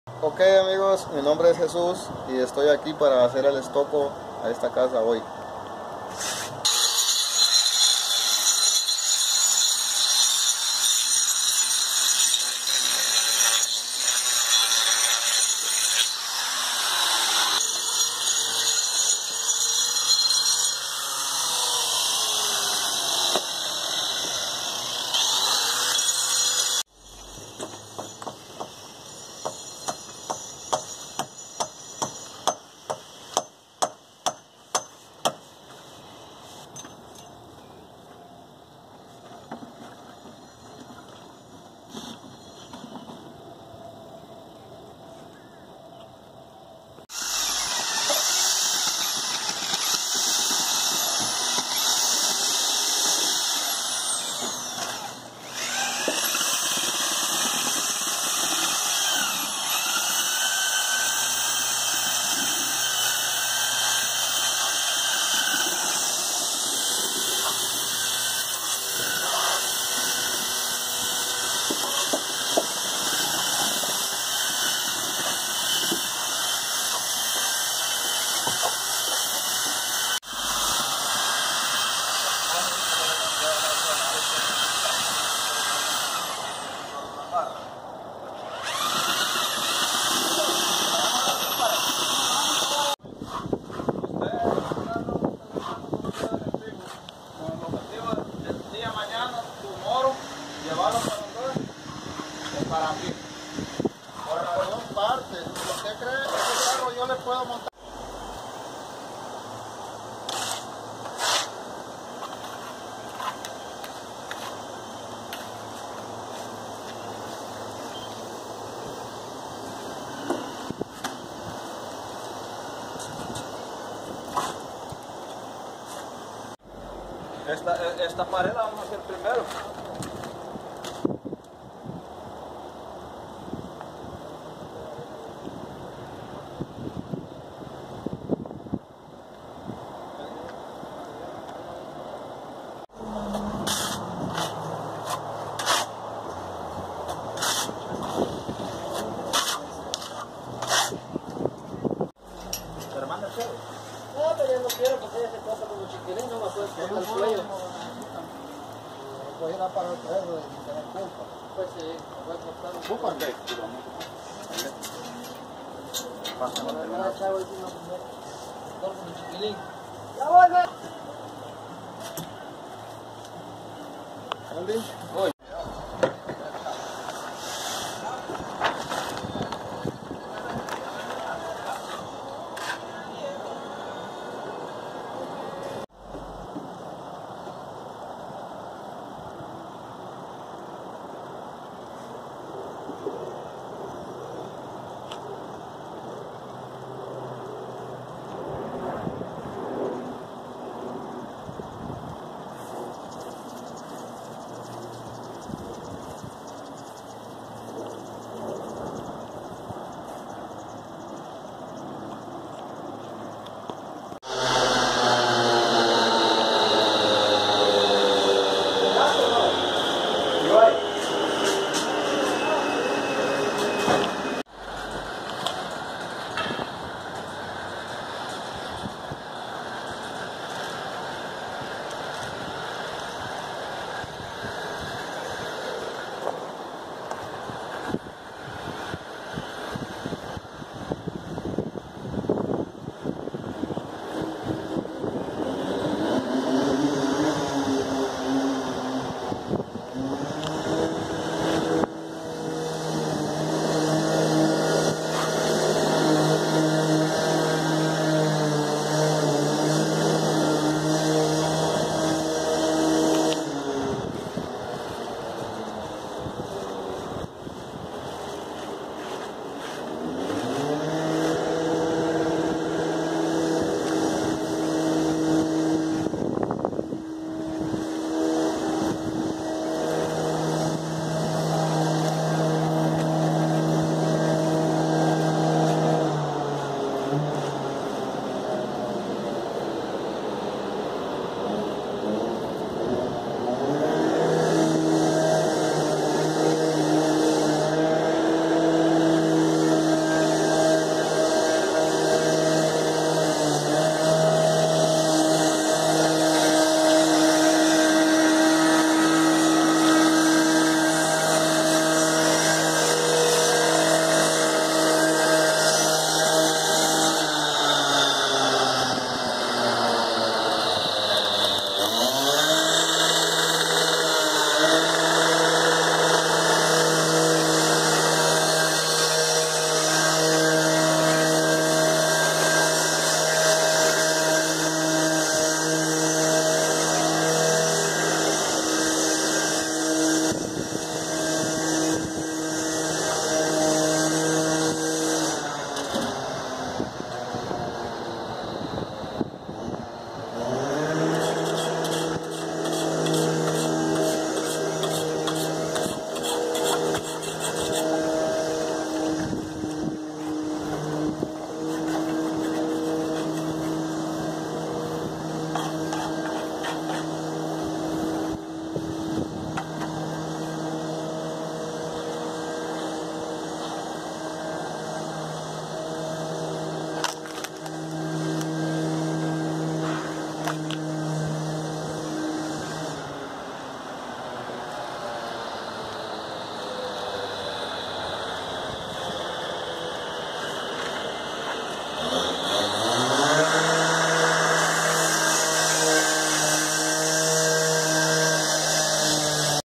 Select Home Ventures LLC - Jesus Ruiz - Designer Stucco
Jesus Ruiz and his crew install stucco on the house. The stucco work includes texture and a two-tone artistic design layout on the front of the house.
Interested in flipping houses? to find help with flipping and before and after pictures of our recent projects.